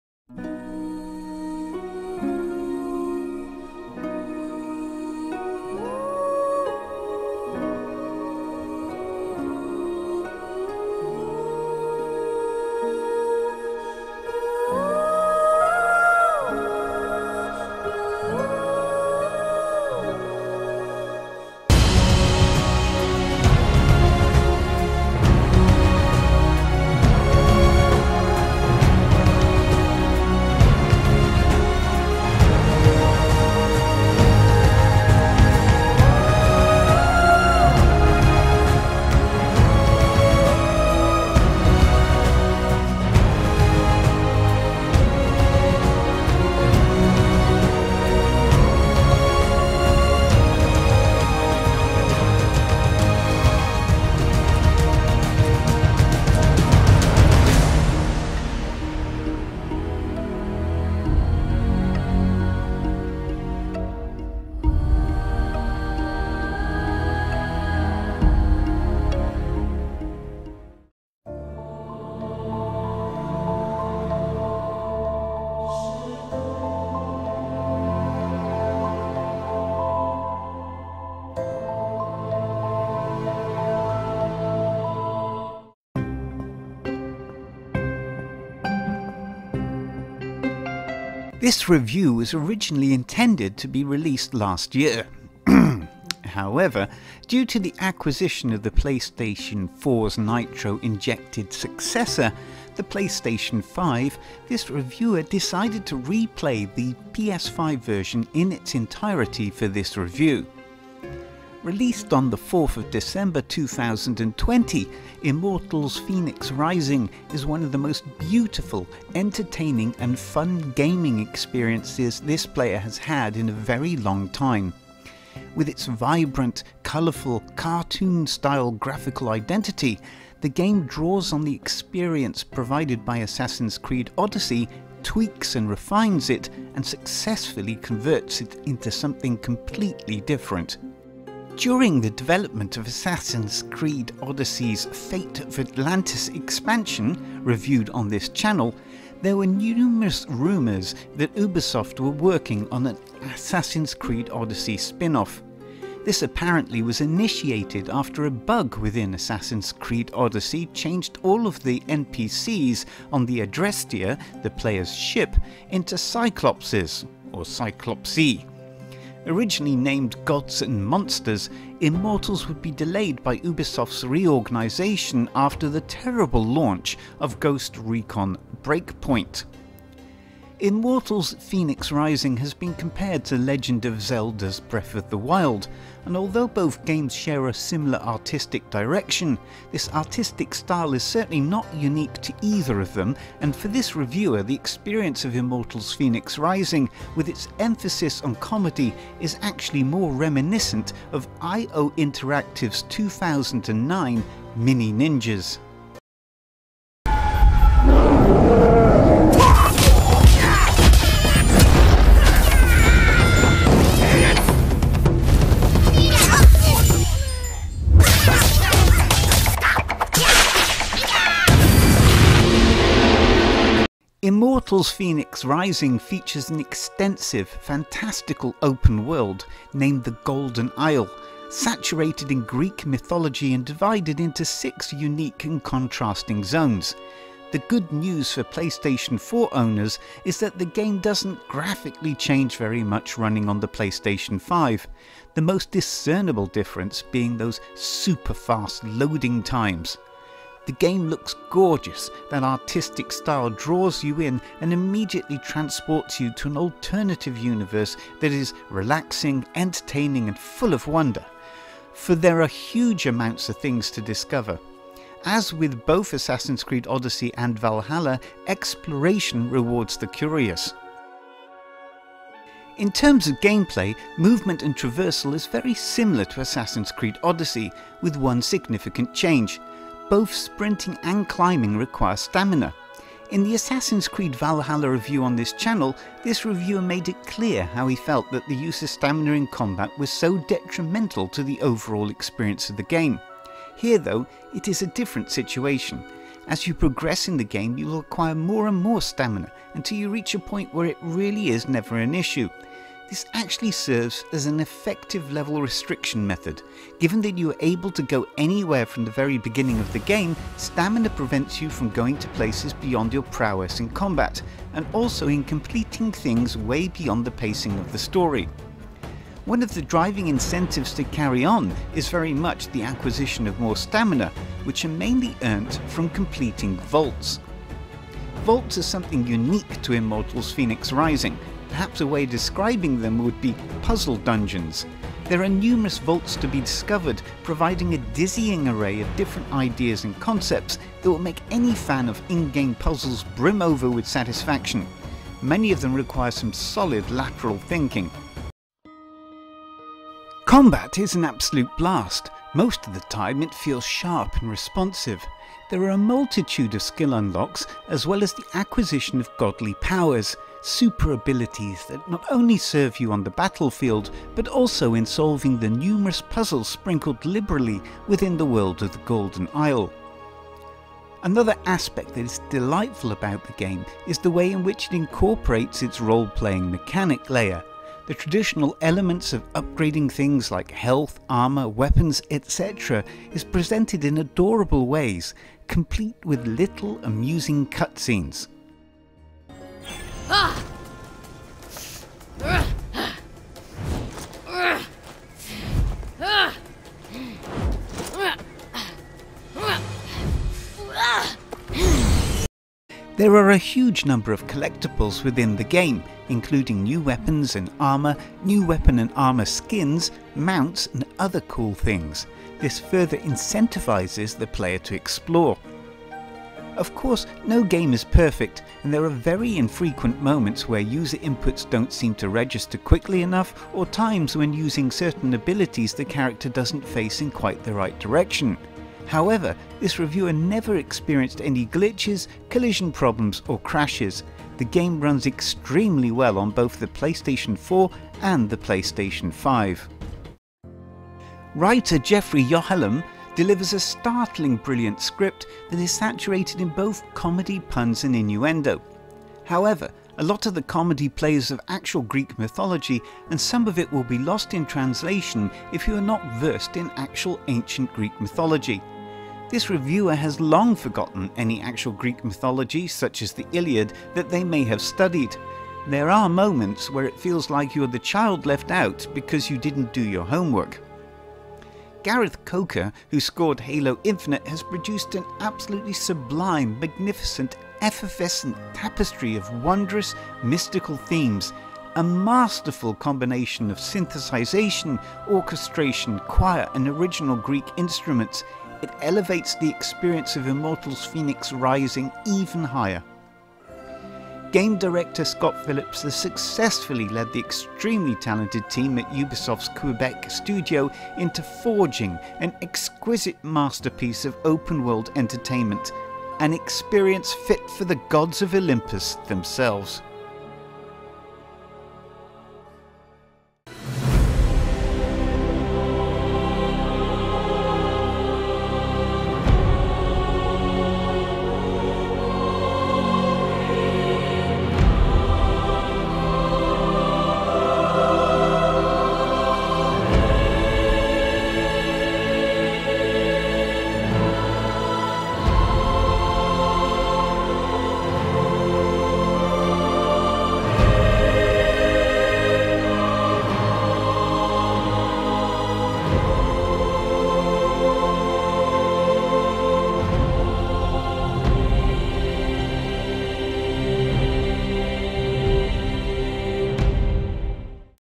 Immortals Fenyx Rising : Review 2023 : AspectGameReviews
Immortals Fenyx Rising is a third-person perspective, comical, action/adventure, open-world, Greek mythology based, role-playing game; that takes place on the imaginary 'Golden Isle'. Immortals Fenyx Rising was developed by Ubisoft Montreal and is published by Ubisoft.

In this video from AspectGameReviews, JCN harnesses his short sword; prepares some dodgy looking smoothie concoctions; constructs some wings out of chicken feathers and barbed wire; and leaps of a cliff, while being followed by a deranged parrot, as he glides into the mystical, wonderfu world of the 'Golden Isle'...